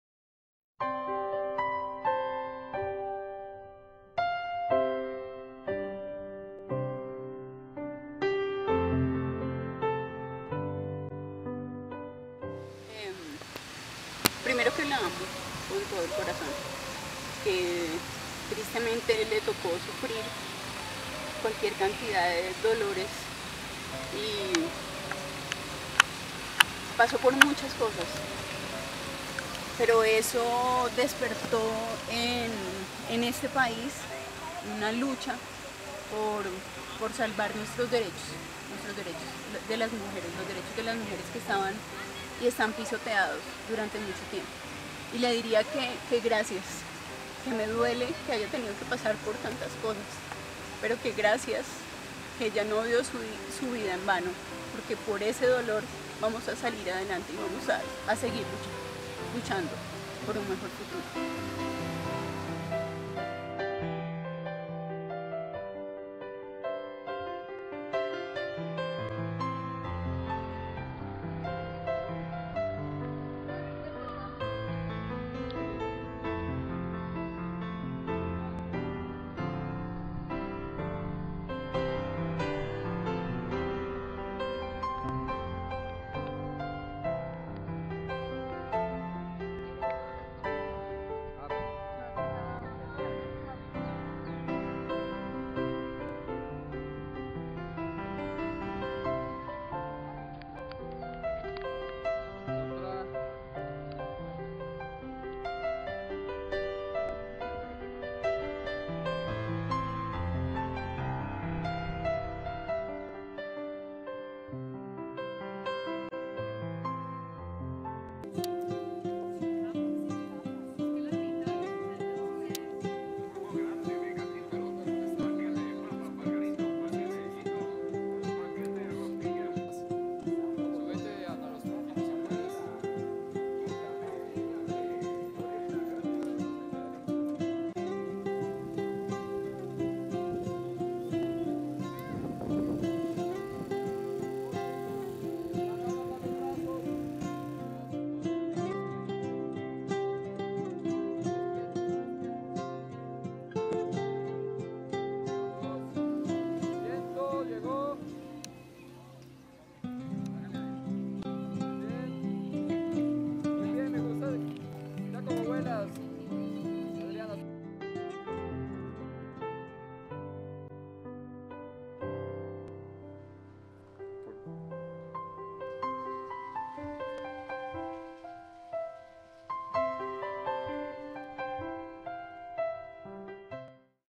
Detras de cámaras. EXPOSICION SOY OBRA DE ARTE.
Exposición Fotográfica en contra del Maltrato de Genero.
Localidad Santa Fe.
Bogotá. 2013